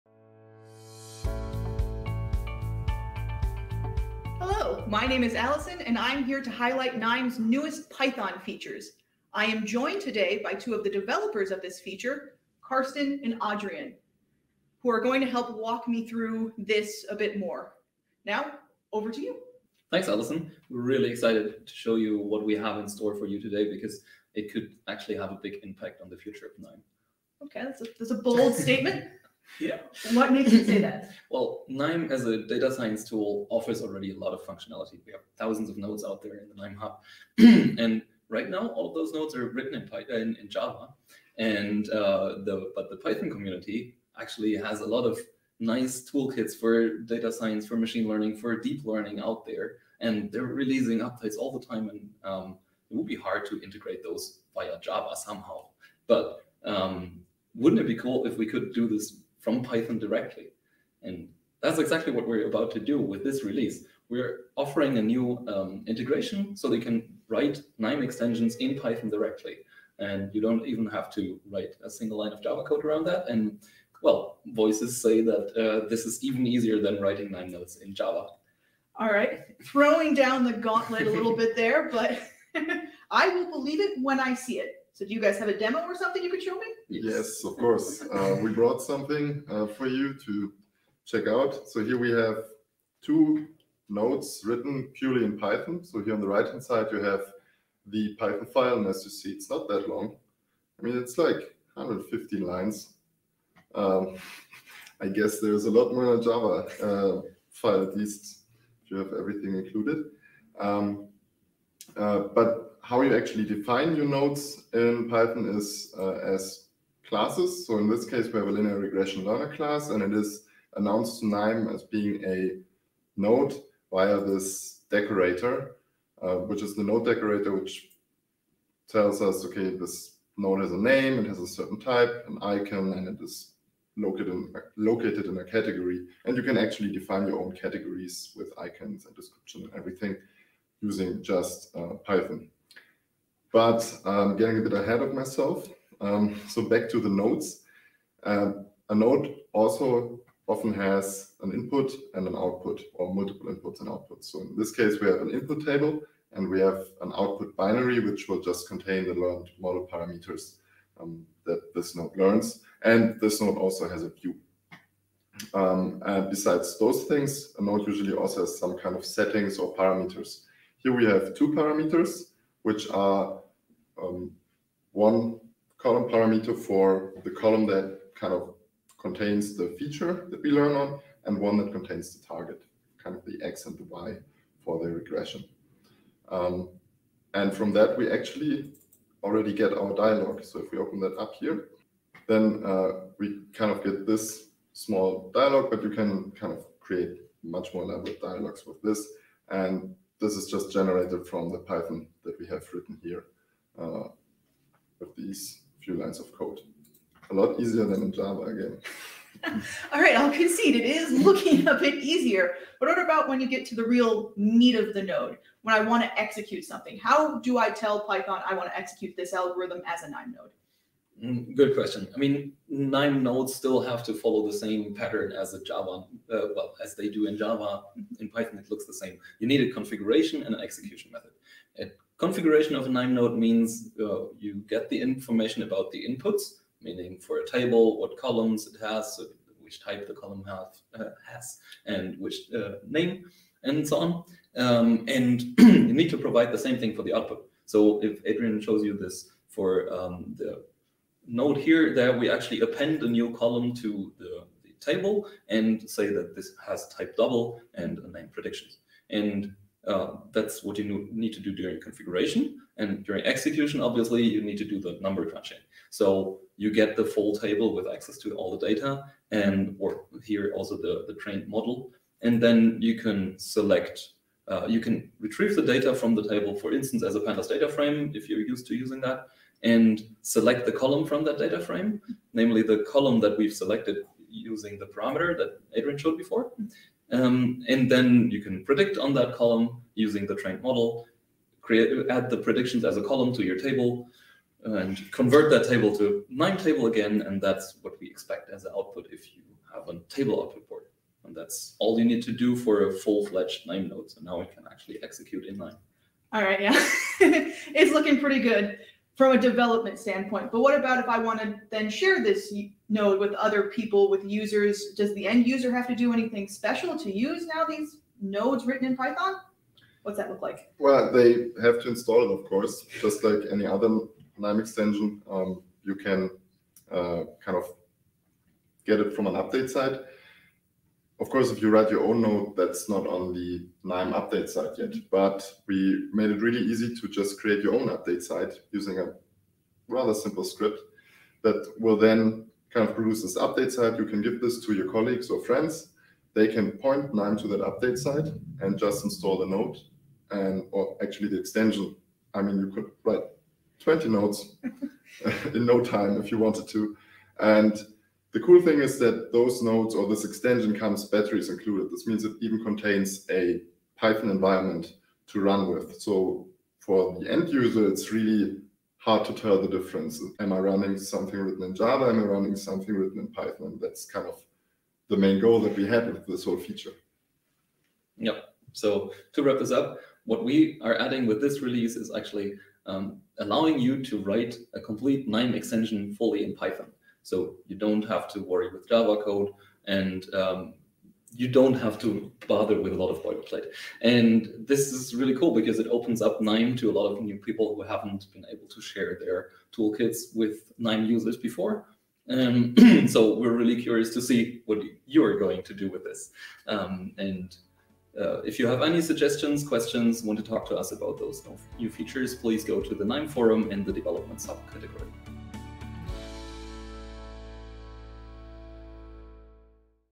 KNIME 4.6: Python
Combine the best of visual programming and scripting with two major updates to the Python Integration. Developers writing custom KNIME nodes can choose to do so completely in Python and easily share these solutions with the team like any other KNIME extension. Better yet, Python environment is built-in. No need to install extra software.